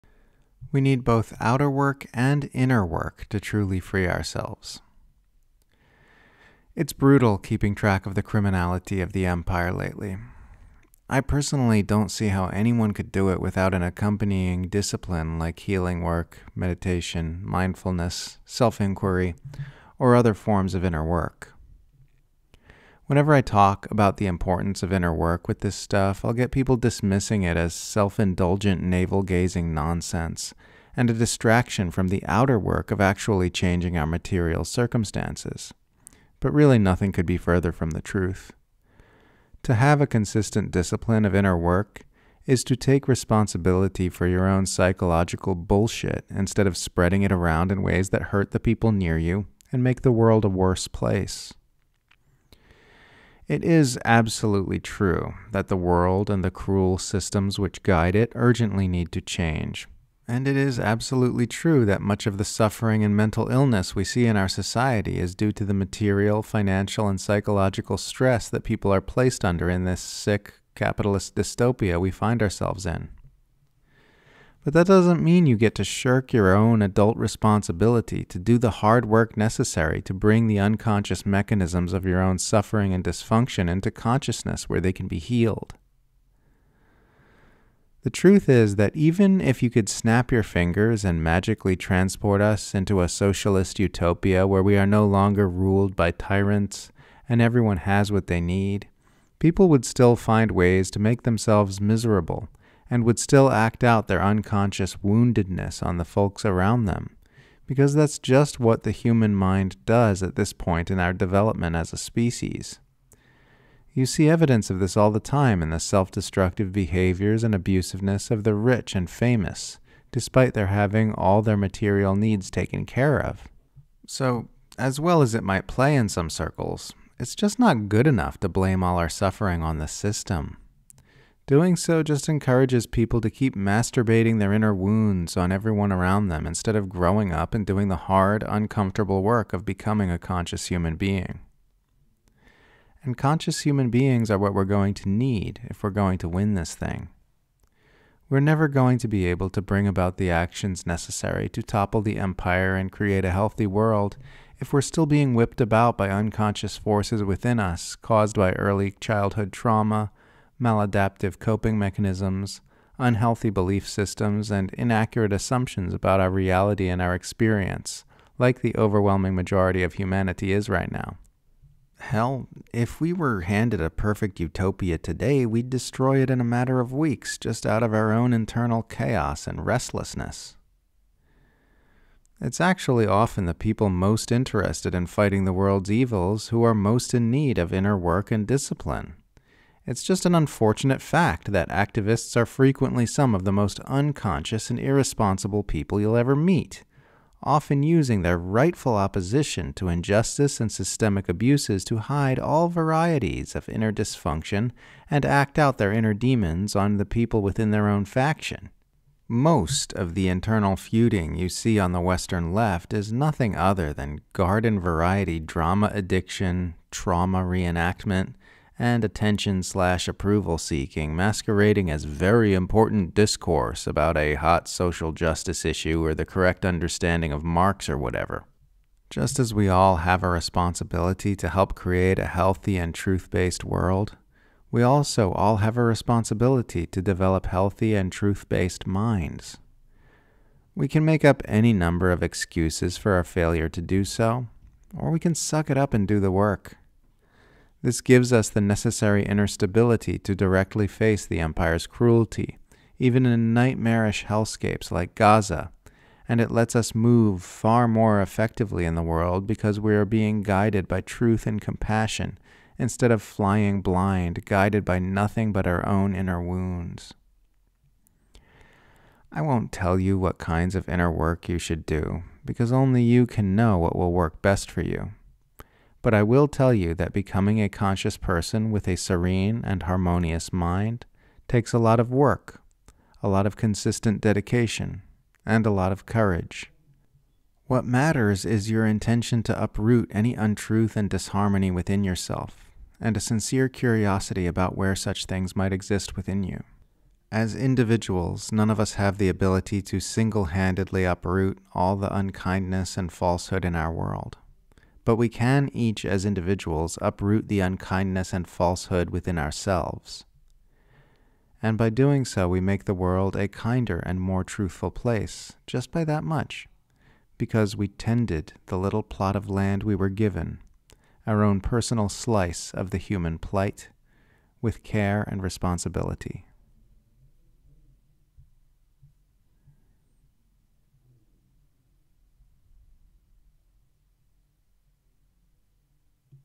We Need Both Outer Work And Inner Work To Truly Free Ourselves
Just as we all have a responsibility to help create a healthy and truth-based world, we also all have a responsibility to develop healthy and truth-based minds.